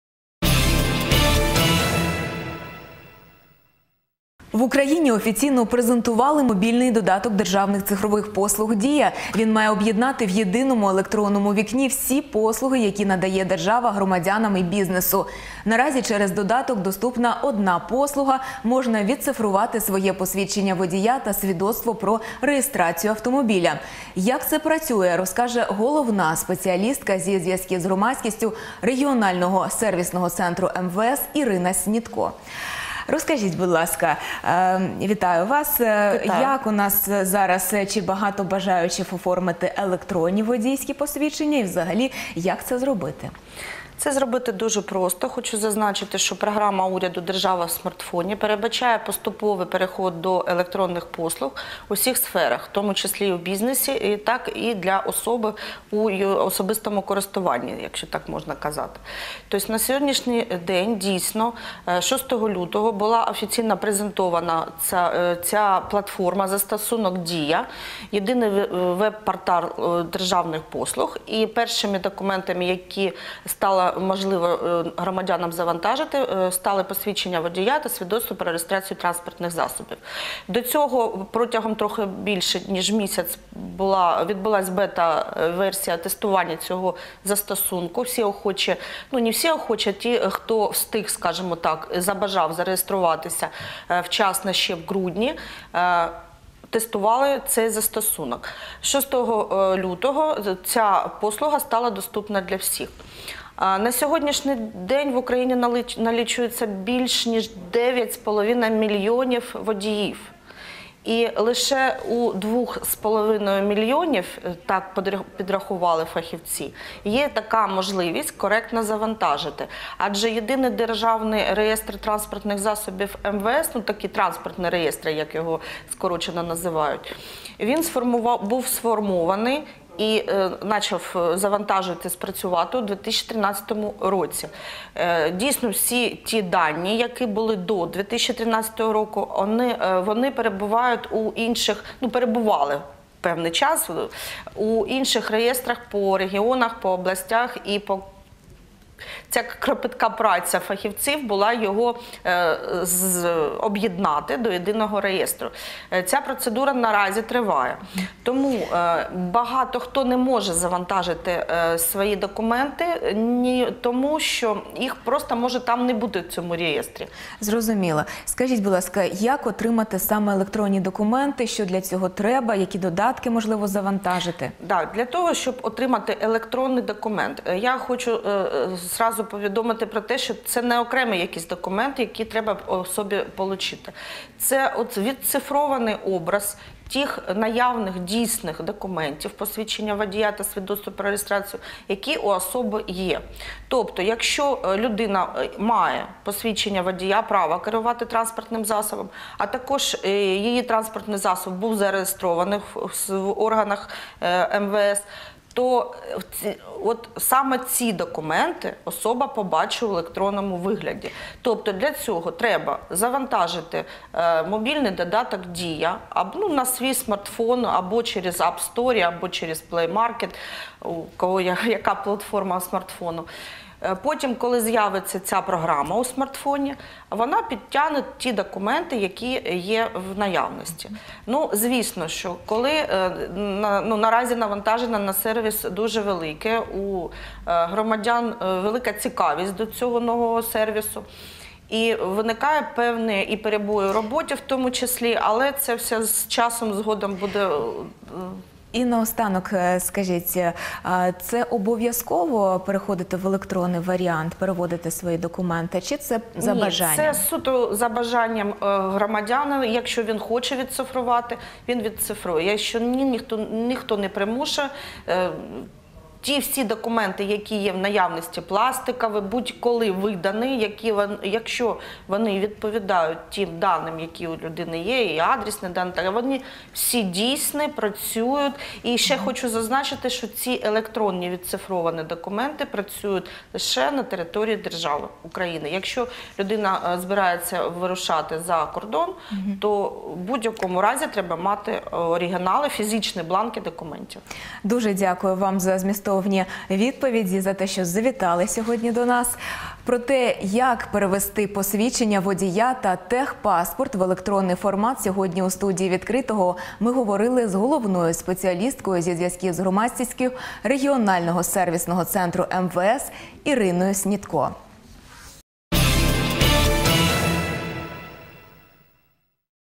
У студії "Відкритого" Ірина Снітько
Як отримати електронне посвідчення водія та техпаспорт, у студії "Відкритого" розповідала головна спеціалістка зі зв’язків з громадськістю регіонального сервісного центру МВС Ірина Снітько________________________________________